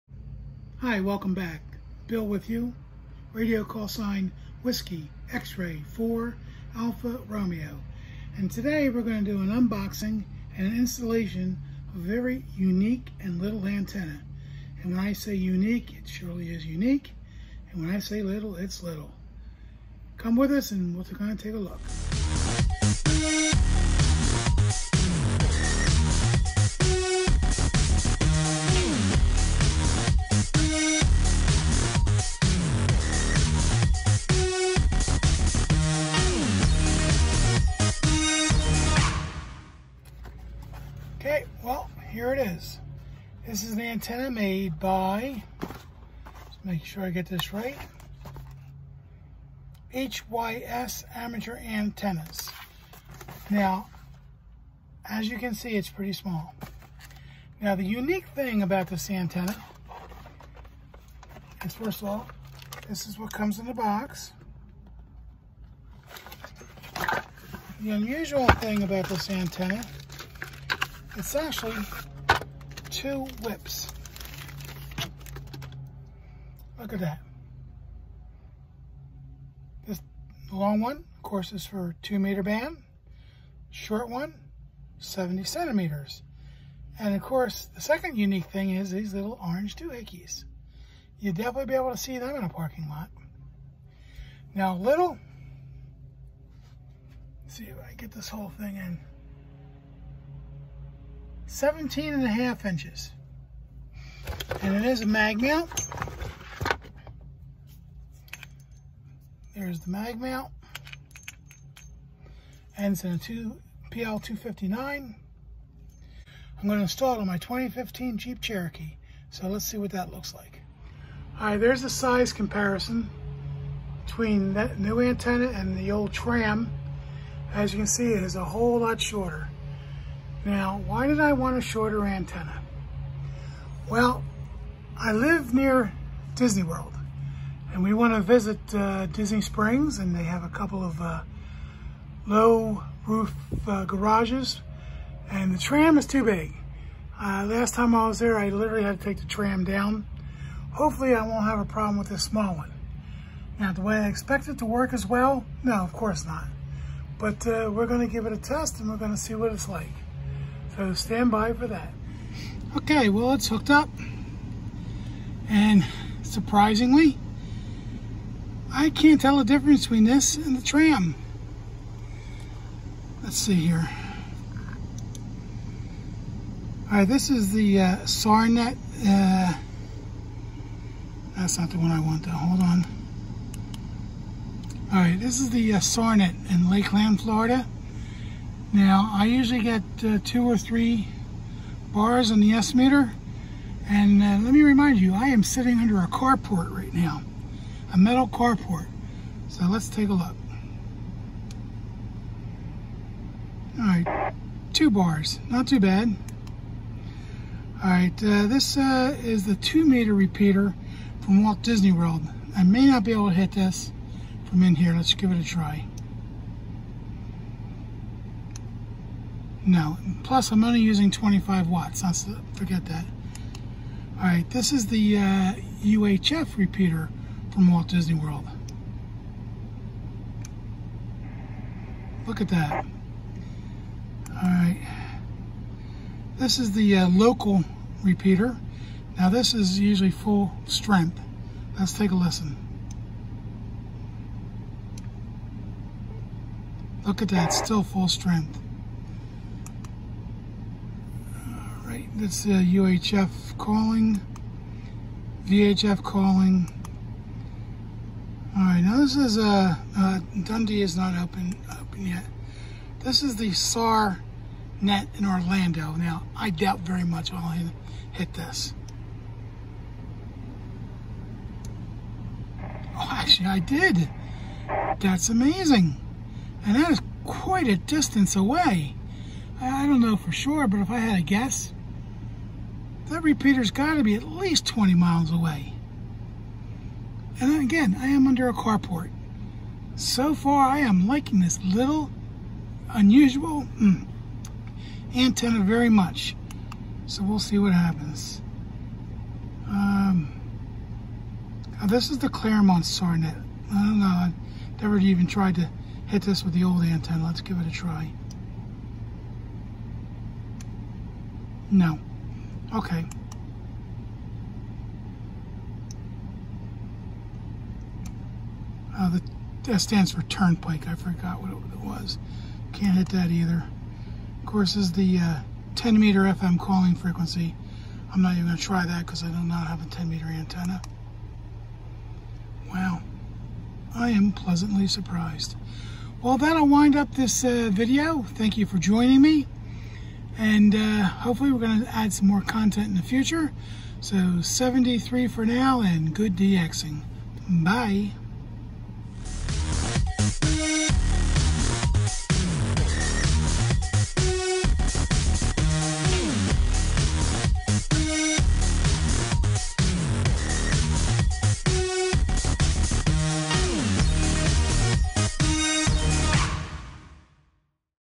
HYS DUAL BAND MOBILE ANTENNA
Unboxing and installation of the HYS Dual Band amateur radio antenna